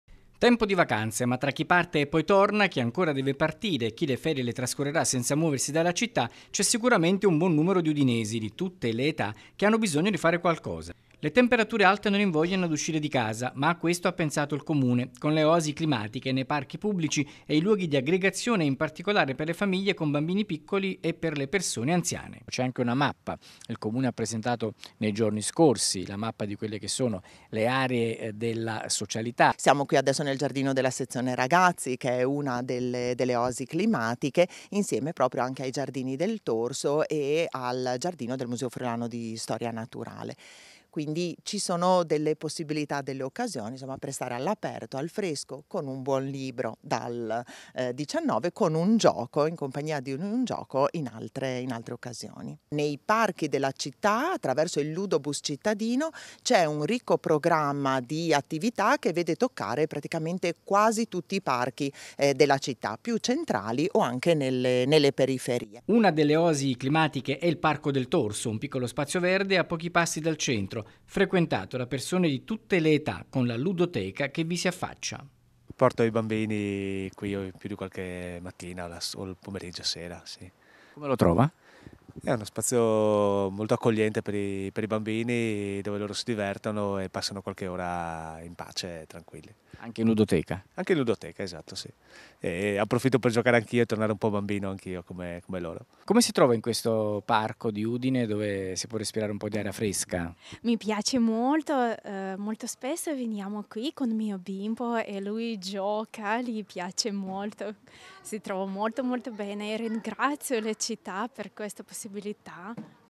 OASI CLIMATICHE, LUDOTECA E BIBLIOTECA JOPPI: LE OPPORTUNITA' PER CHI RESTA IN CITTA' | 06/08/2024
TG Udine 06/08/2024 - UDINE - Temperature alte e poca voglia di uscire di casa, ma chi resta a Udine può contare su spazi verdi freschi e areggiati e luoghi di aggregazione e attività, in particolare per famiglie e persone anziane. - Intervistati CRISTINA MARSILI (RESPONSABILE SERVIZIO BIBLIOTECHE) (Servizio di Francesco Pezzella)  - Segui TV 12 anche sul digitale terrestre! Visita il sito tv12.medianordest.it